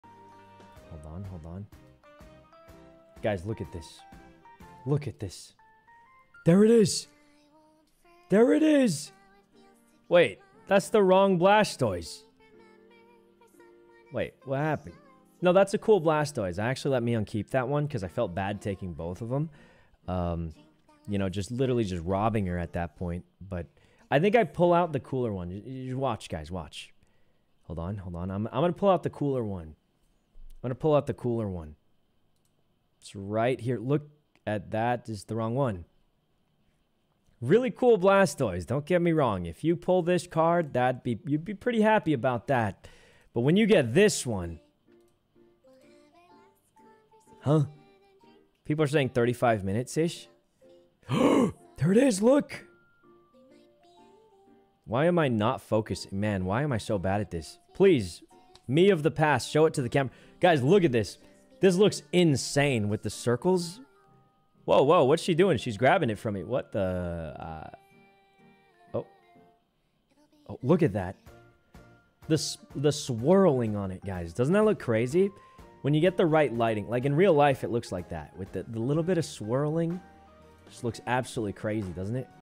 Sykkuno cosplay, showing the stream VOD when he pulled THE CARD.
Follow Sykkuno!